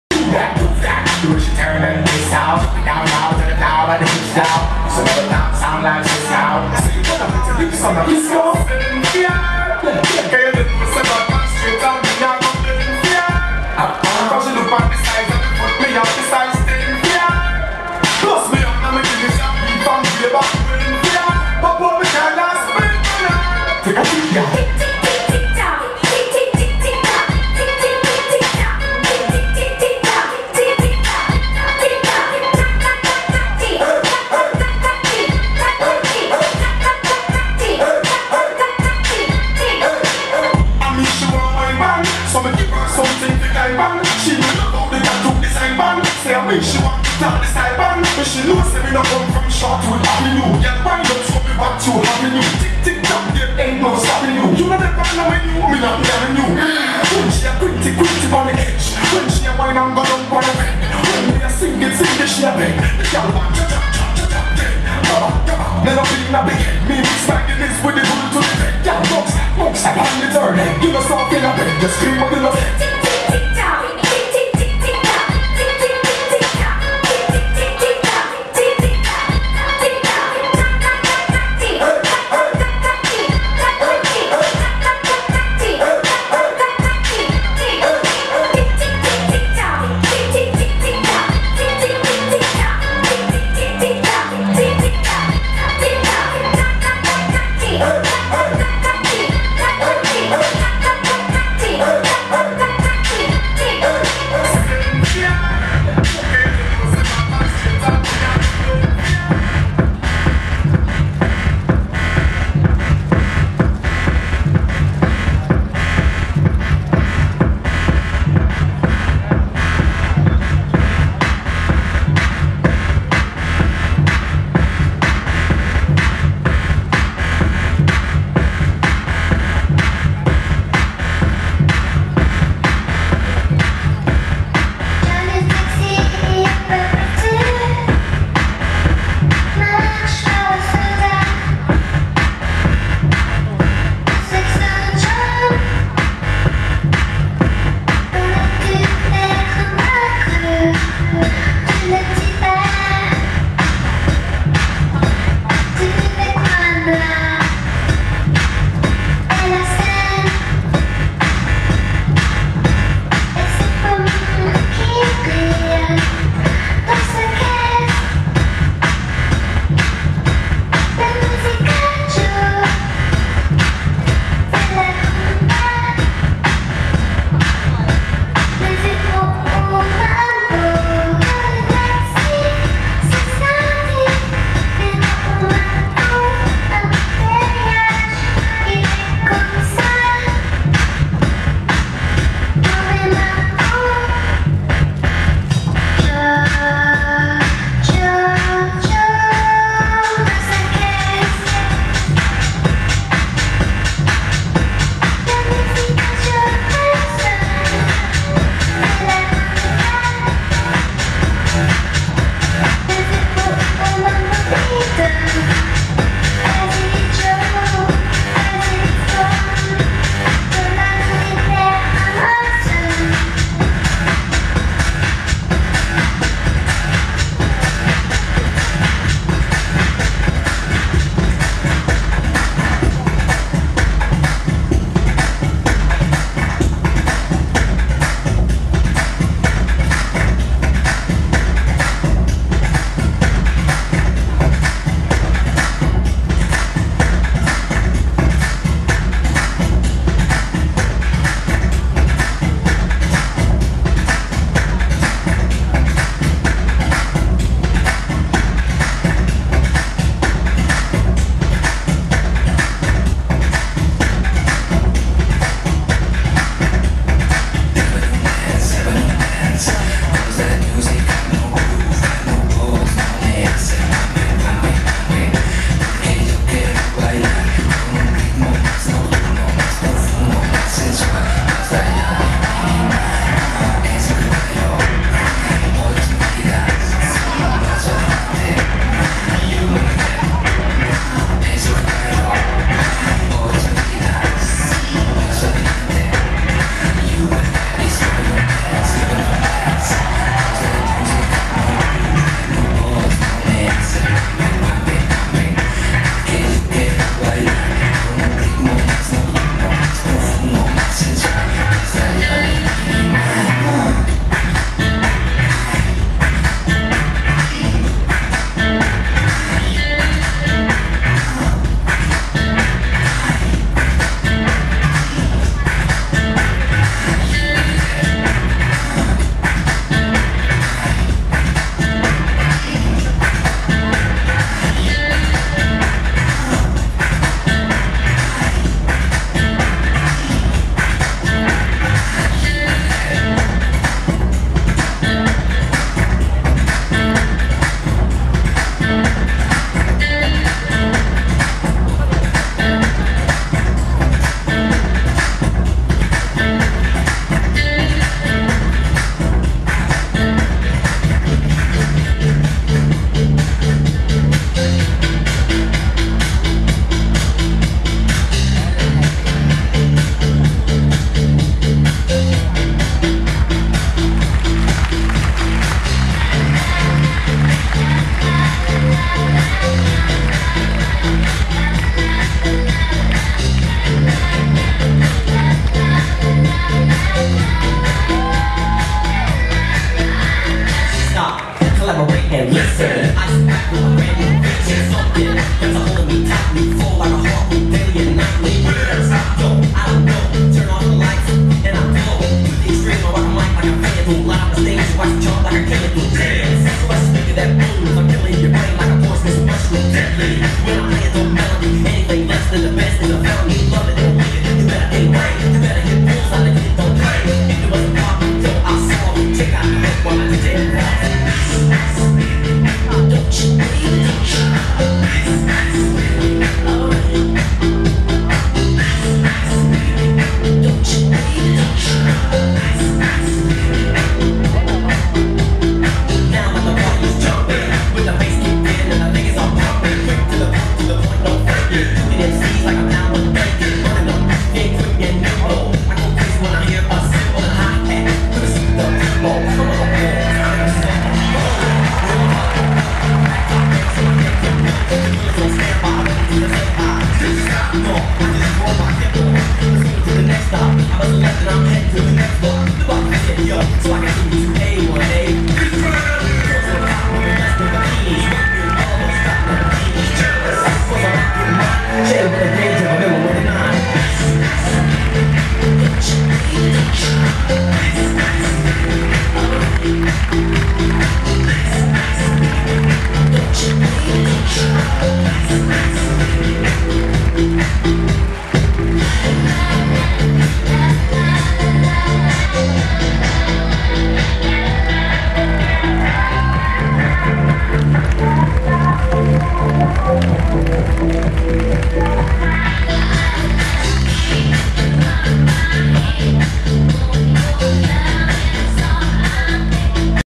27Nov 2/2- BIFW08AW
Bangkok International Fashion Week 2008 Autumn/Winter, 20-23 November 2008 @ Siam Paragon